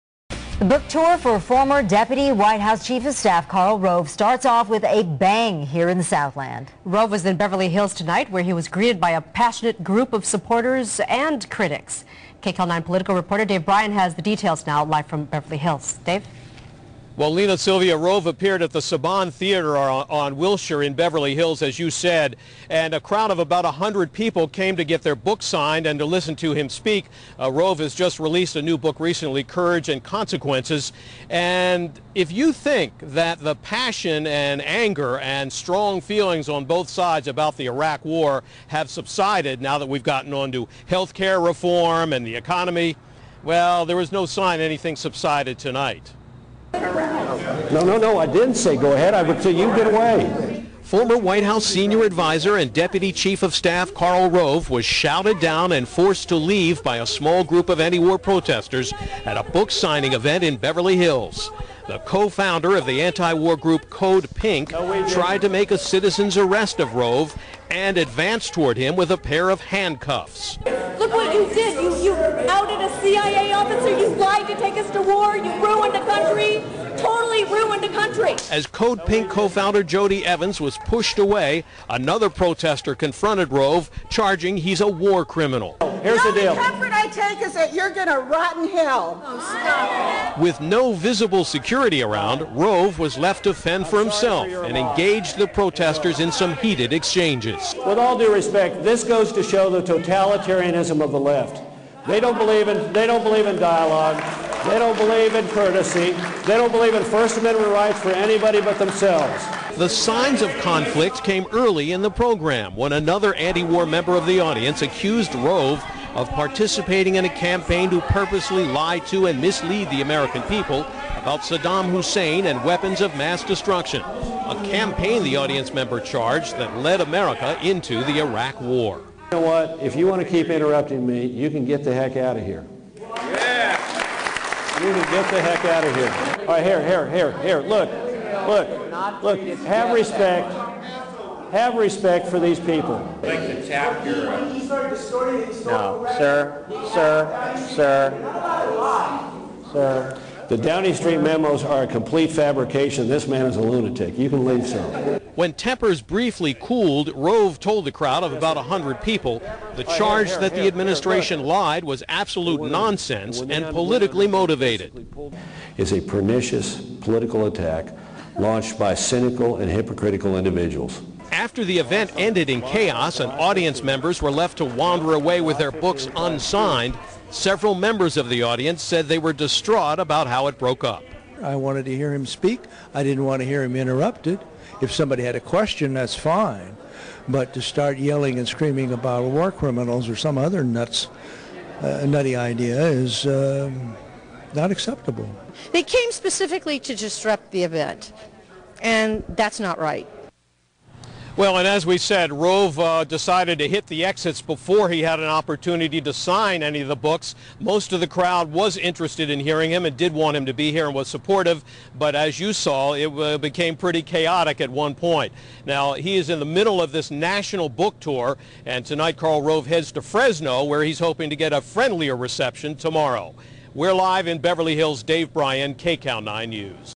Karl Rove Flees Code Pink, Ditches Own Book-Signing Event in Beverly Hills
CodePink co-founder Jodie Evans approaches Karl Rove with handcuffs, attempts to make citizen's arrest.

"Look what you did. You outed a CIA officer, you lied to take us to war, you ruined a country.  You totally ruined a country!" -- Jodie Evans, co-founder, CodePink

"The Downing Street memos are a complete fabrication. This man is a lunatic." -- Karl Rove in response to another protester

Never Forget: "Bush wanted to remove Saddam, through military action, justified by the conjunction of terrorism and WMD. But the intelligence and facts were being fixed around the policy." -- The Downing Street Memo, July 23rd, 2002